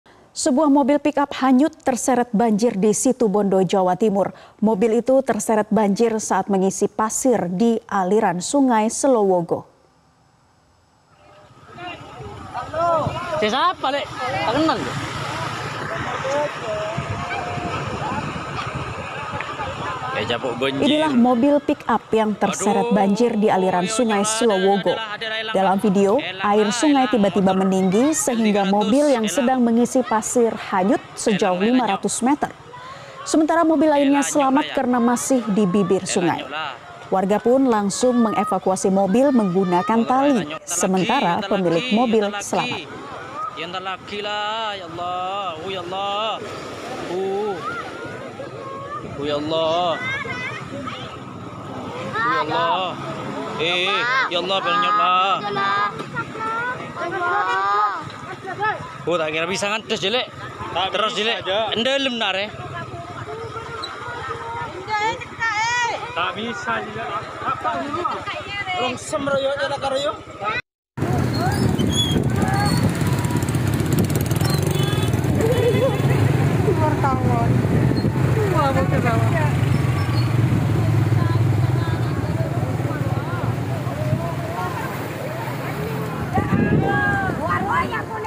Mobil Pick Up Hanyut Terseret Banjir di Jawa Timur
MetroTV, Sebuah Mobil Pick Up hanyut terseret banjir di Situbondo, Jawa Timur. Diketahui mobil tersebut sedang mengisi pasir di sungai Selowogo.

namun debit air tiba - tiba naik dengan cepat dan langsung menyeret mobik tersebut, diketehui mobil terseret arus sepanjang 500 meter.

Diketahui, ada beberapa mobil lain namun semuanya selamat karena masih berada di bibir sungai.